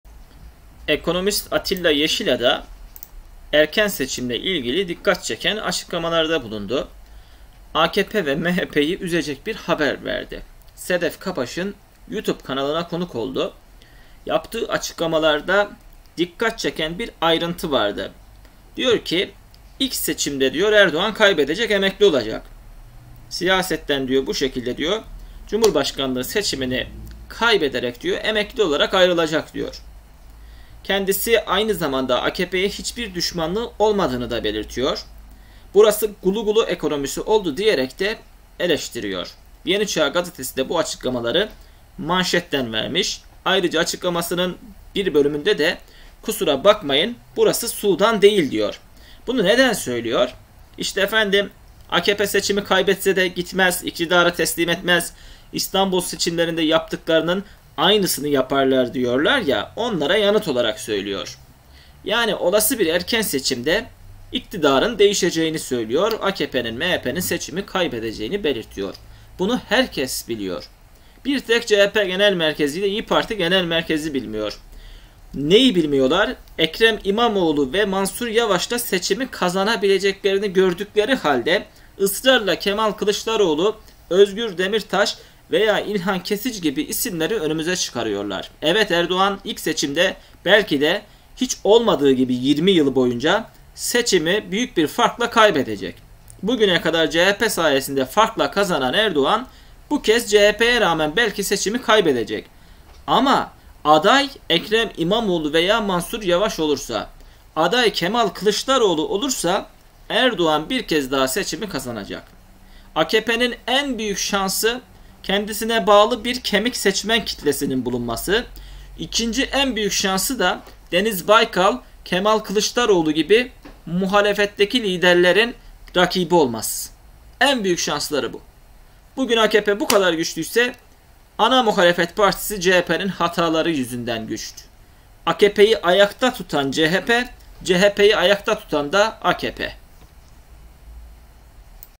Ünlü Ekonomist İktidarın değişeceği tarihi açıkladı
Ekonomist Atilla Yeşilada, Sedef Kabaş'ın Youtube kanalına konuk olarak iktidarla ilgili şaşırtıcı iddialarda bulundu.